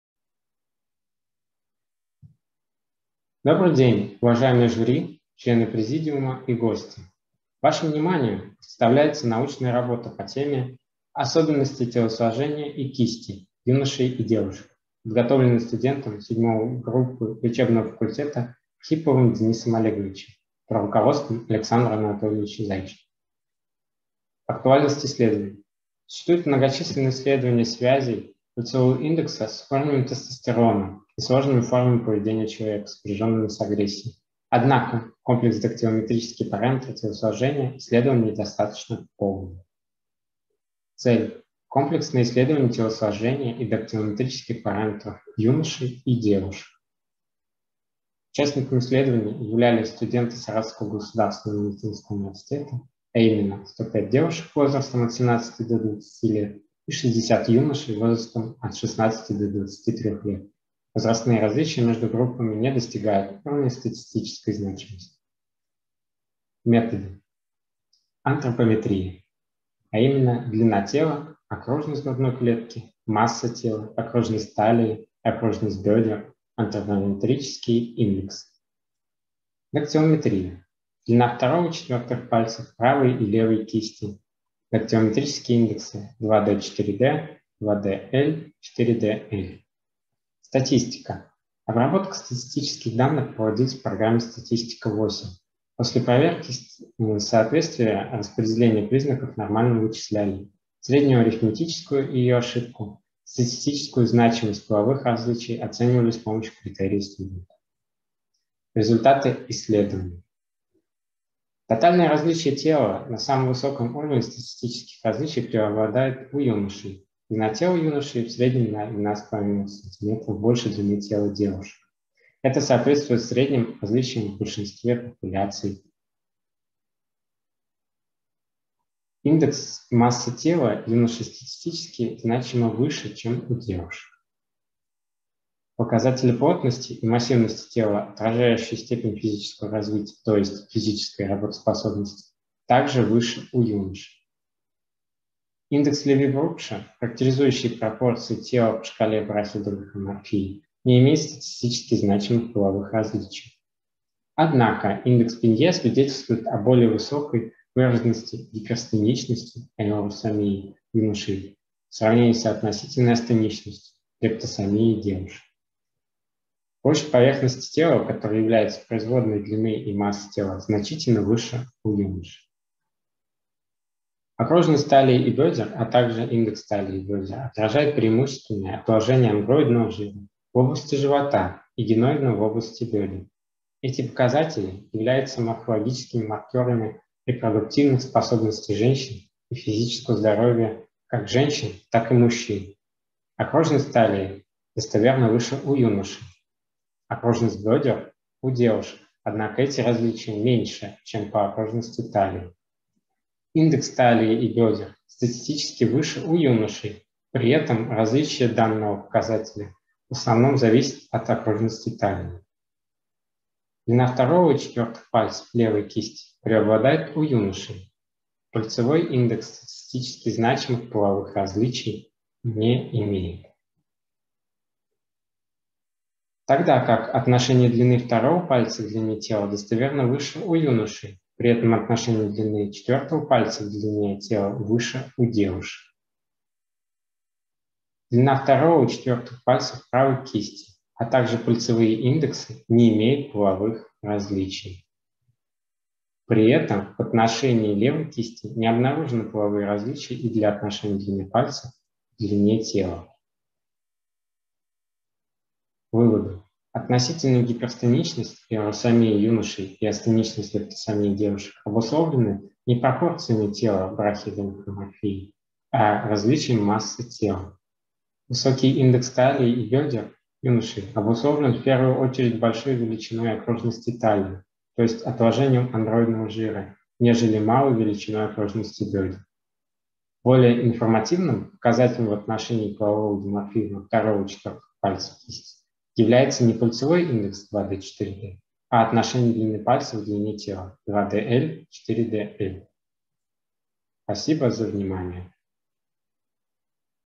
ОСОБЕННОСТИ ТЕЛОСЛОЖЕНИЯ И КИСТИ (2D:4D) ЮНОШЕЙ И ДЕВУШЕК
Архипов Денис Олегович – студ. 2 курса леч. фак. СГМУ им. В.И. Разумовского.
Научный руководитель: Зайченко Александр Анатольевич, д-р мед. наук, профессор кафедры анатомии человека СГМУ им. В.И. Разумовского.